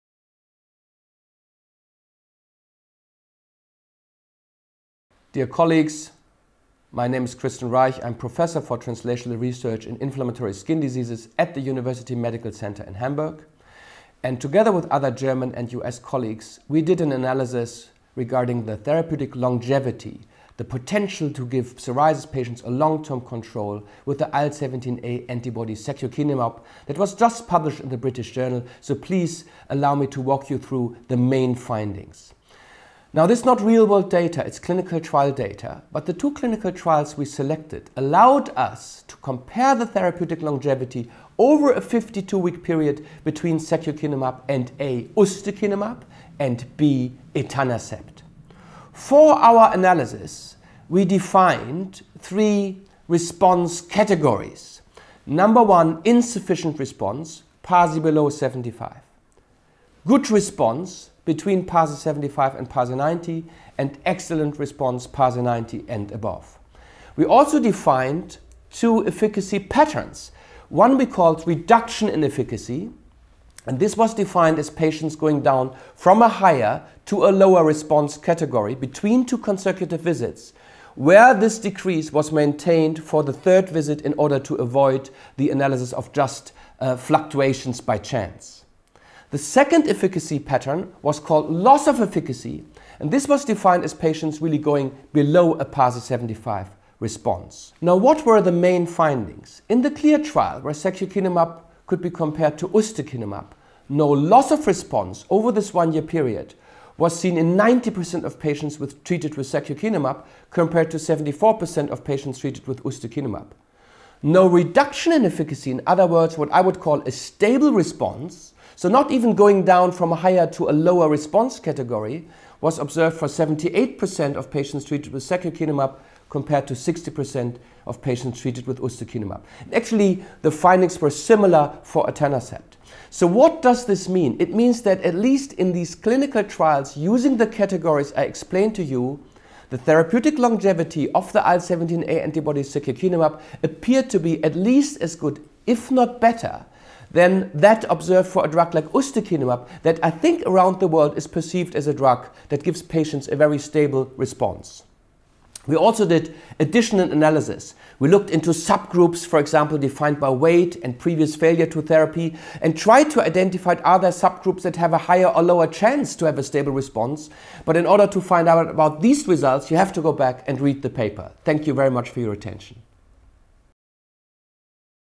Stability of response to secukinumab over 52 weeks, M. Augustin et al
Author video for 'Continued treatment with secukinumab is associated with high retention or regain of response'